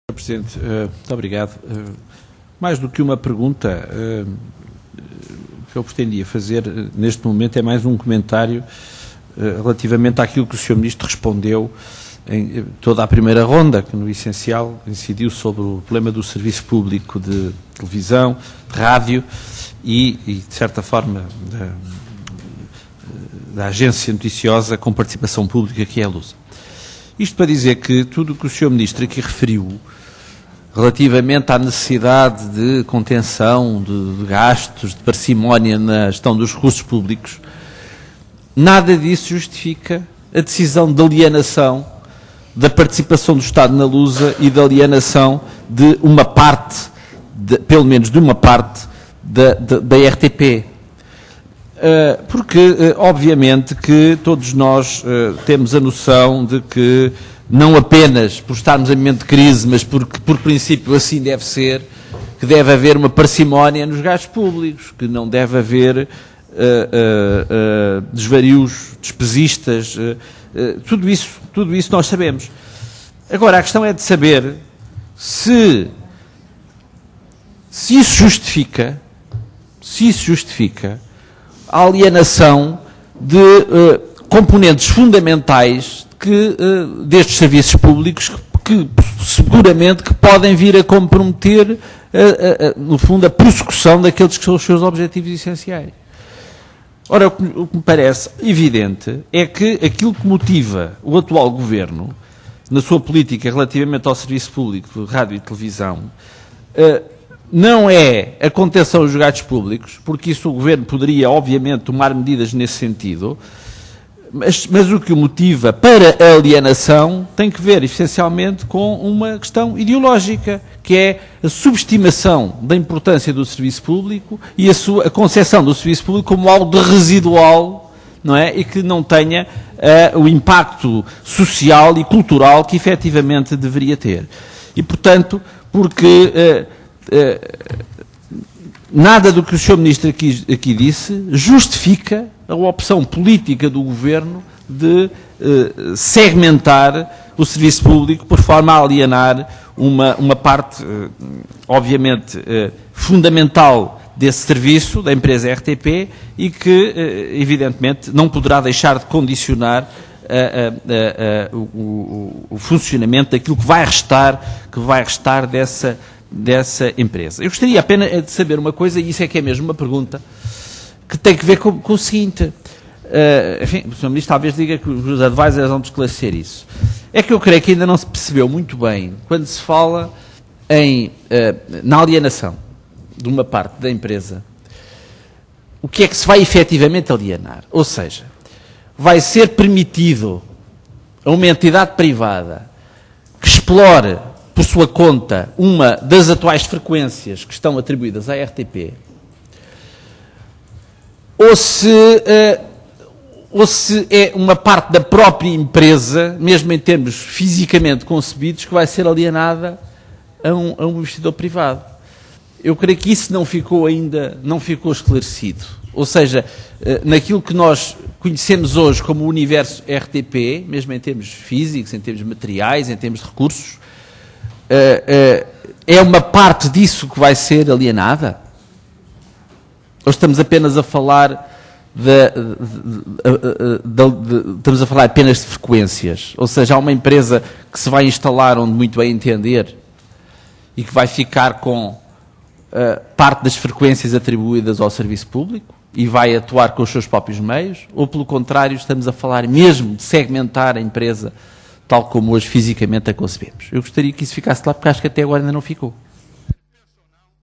O governo não tem justificações para a privatização da RTP e da Lusa
António Filipe, PCP, Comissão para a Ética, a Cidadania e a Comunicação, 10.7.012